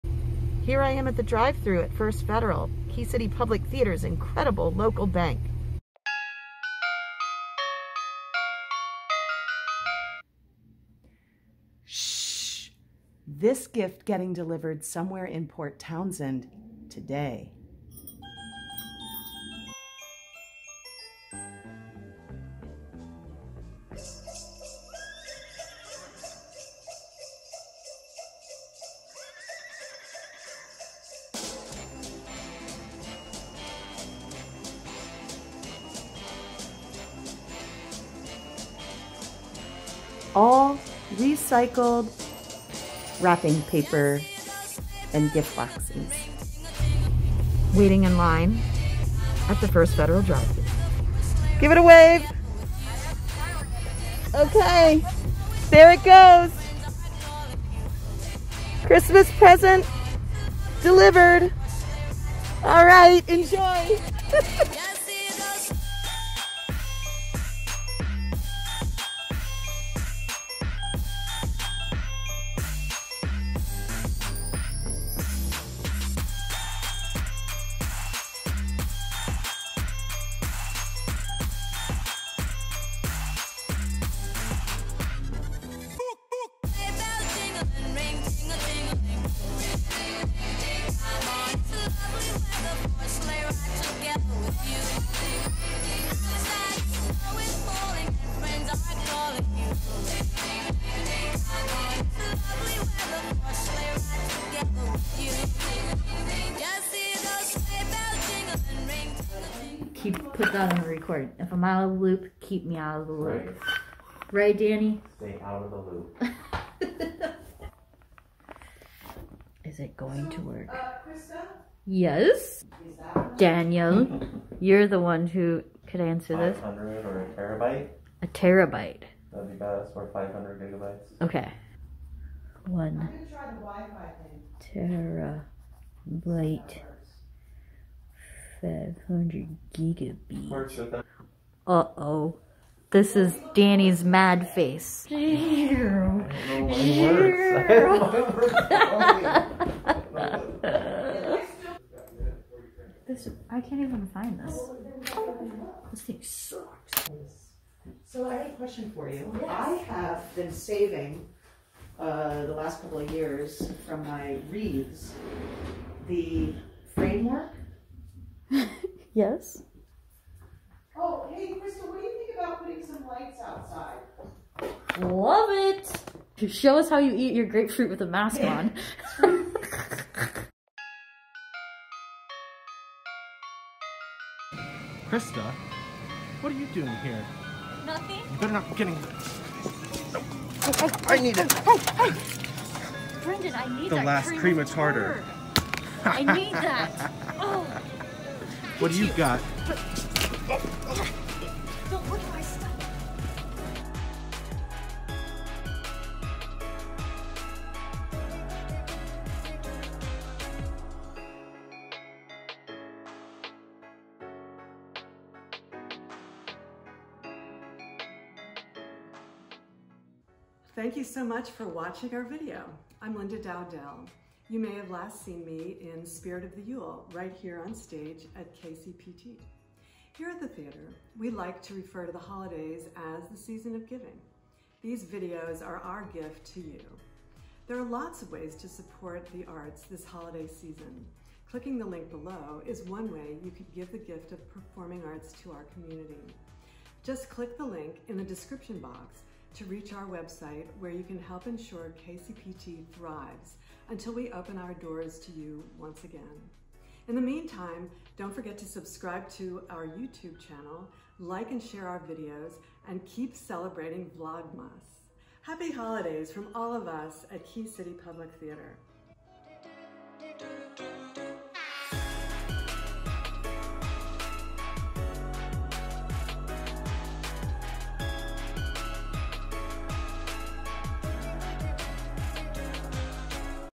KCPT VLOGMAS DAY 16
Denise delivers a handmade gift while Christa jokes around with everyone in the office.